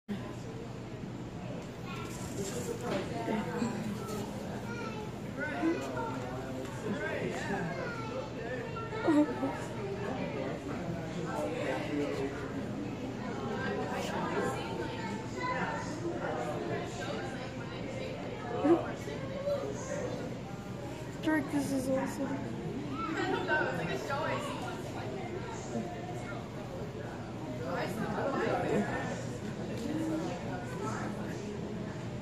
A WINDOW LICKING HAMSTER 😯 🤔😛😆😂
when you go to buy your kids a pet and find the only window licking hamster in the store